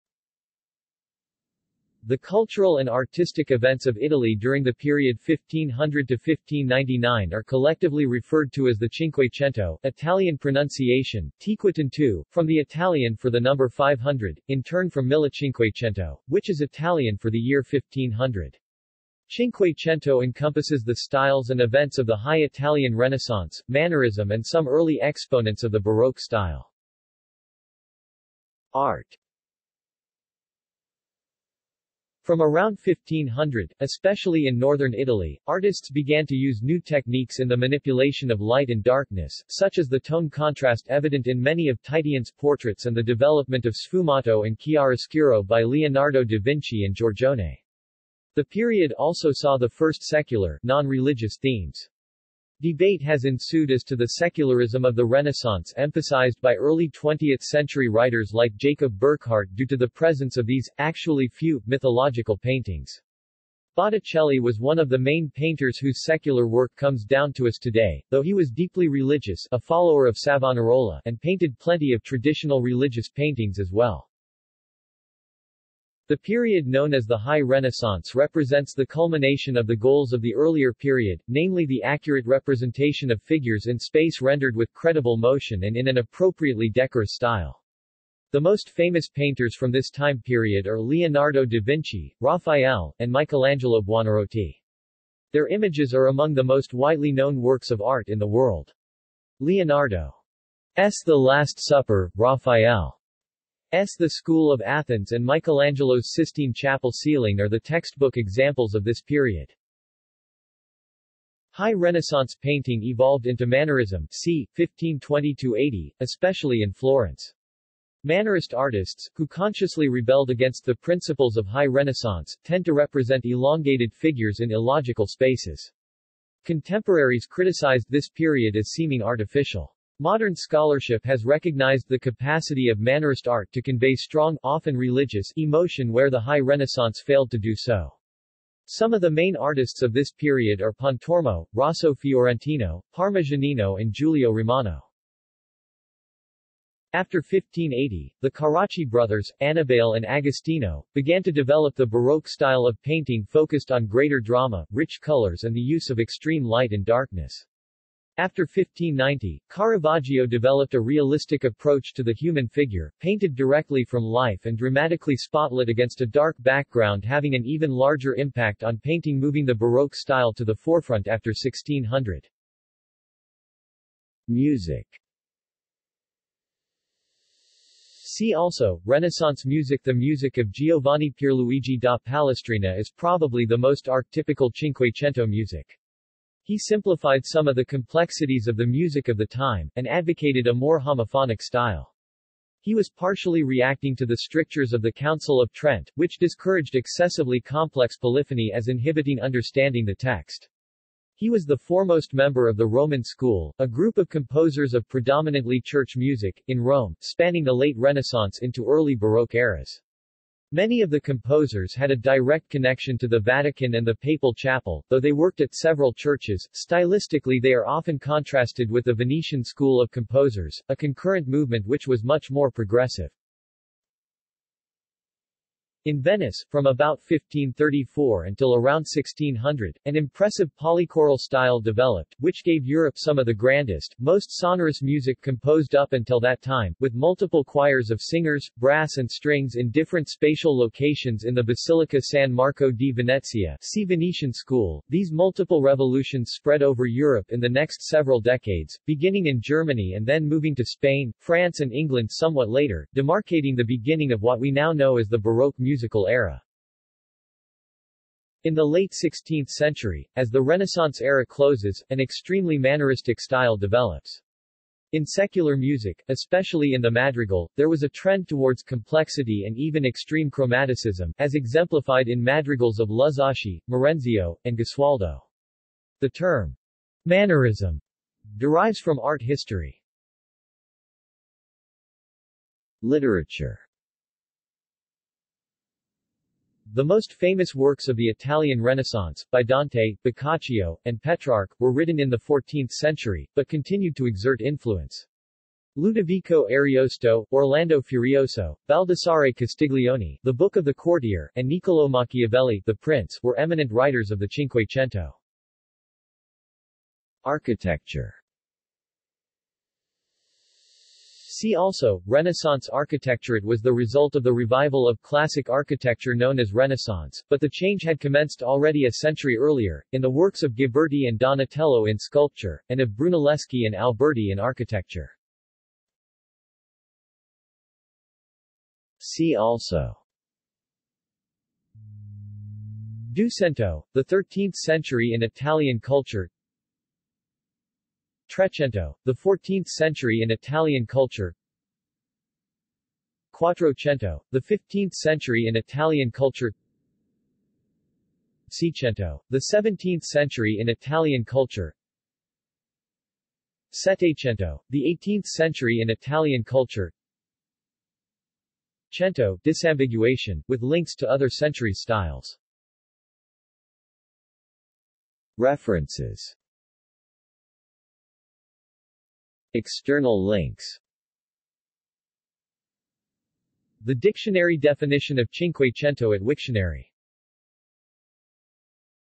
Cinquecento | Wikipedia audio article
This is an audio version of the Wikipedia Article:
Cinquecento

SUMMARY
The cultural and artistic events of Italy during the period 1500 to 1599 are collectively referred to as the Cinquecento (Italian pronunciation: [ˌtʃiŋkweˈtʃɛnto]), from the Italian for the number 500, in turn from millecinquecento, which is Italian for the year 1500. Cinquecento encompasses the styles and events of the High Italian Renaissance, Mannerism and some early exponents of the Baroque-style.